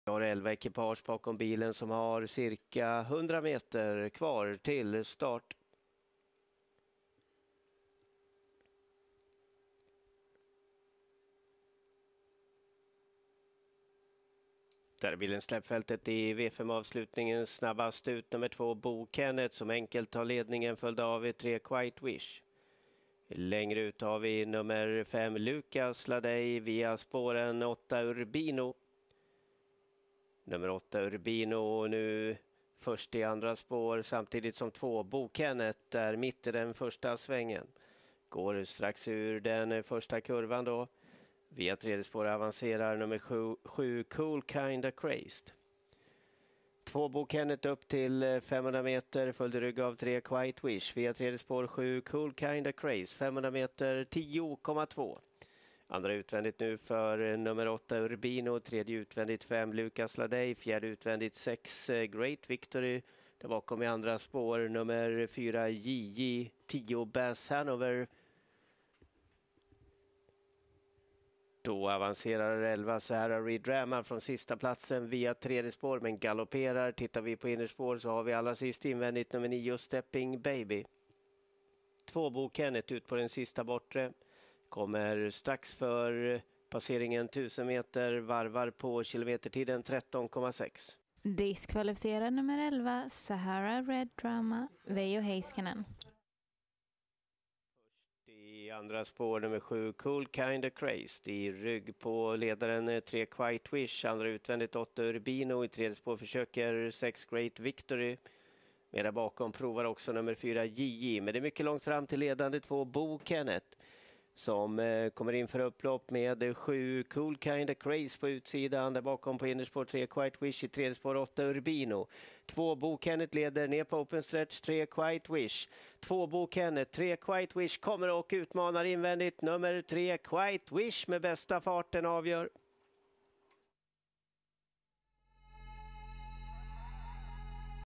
Åbytravet170921-Lopp9
Lopp 9. Nostalgiloppet Ljus anmälan. 3-åriga och äldre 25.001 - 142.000 kr.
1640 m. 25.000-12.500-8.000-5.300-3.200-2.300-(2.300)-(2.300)
1.Quite Wish 12,7 (143) Eriksson K
2.Bo Kenneth 12,8 (14) Untersteiner P
3.Dalforno 13,2 (91) Eklundh A
4.Coolkindacrazed 13,2 (161) Ingves P
5.Lucas Laday 13,4 (252) Sjunnesson J
6.J.J. 13,7 (67) 7. Bas Hanover 13,8 (1059) Opl. Urbino 13,8 (886),
Great Victory 14,1 (516), Stepping Baby 19,0 (265), Sahara Redrama* d11g (129).
Struken: Ramon Dekkers.
Segr. h 4 år e. Quite Easy U.S.-Jetta Lady.
Äg-Uppf. Alma Trav AB, Eslöv. Trän. Stenströmer Ulf, Skara. 0,5 längd.
Odds: 14,36. Plats: 2,56-1,32-1,96. Tvilling: (2/3) 13,10.